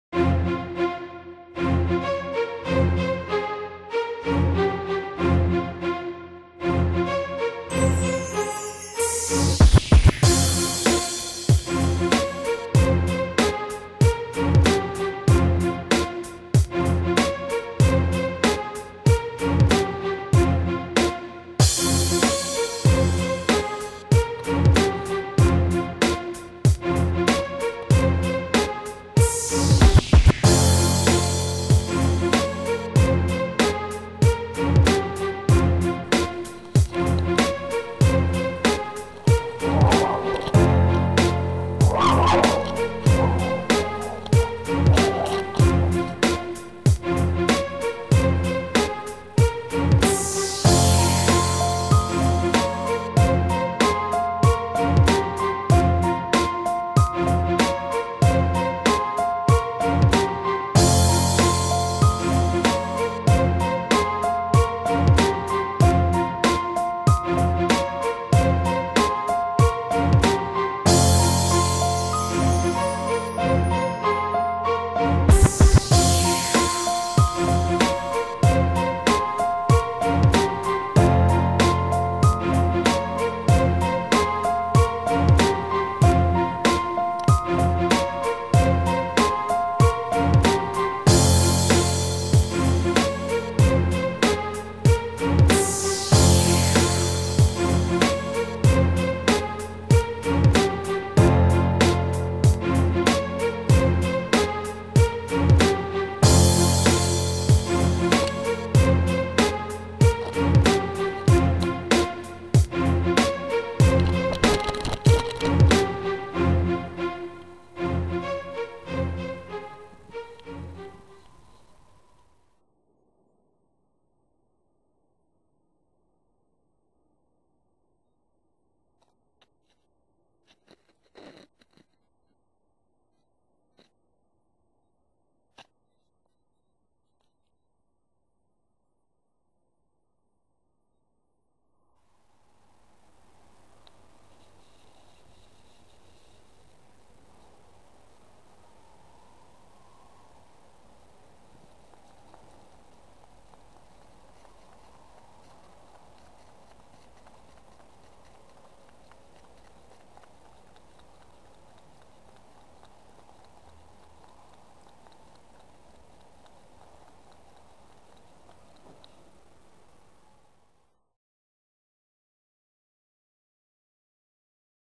un chat avec une caméra embarquée
chat qui part en mission, croise un chien, monte sur le toits...